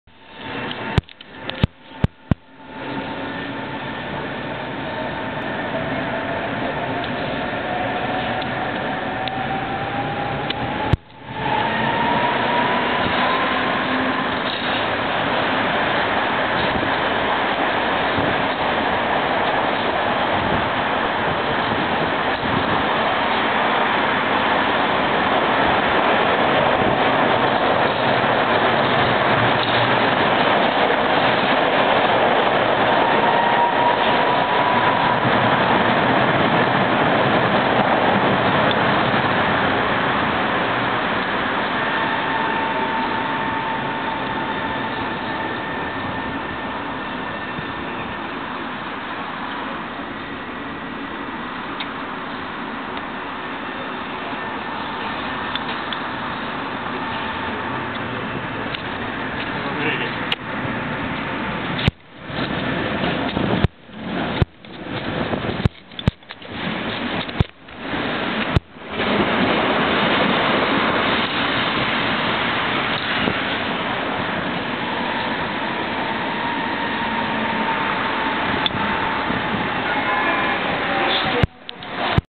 Jízda tramvají T3R.P mezi zastávkami Na Veselí a Pražského povstání
Jízda tramvají T3R.P vozovny Pankrác, která byla vypravena jako náhrada za tramvaj Škoda 15T evidenčního čísla 9203 vozovny Pankrác mezi zastávkami Na Veselí a Pražského povstání na lince 18 ve směru Petřiny.(U zastávky Pražského povstání projela tramvaj Škoda 15T na zkušební jízdě) 5.3.2011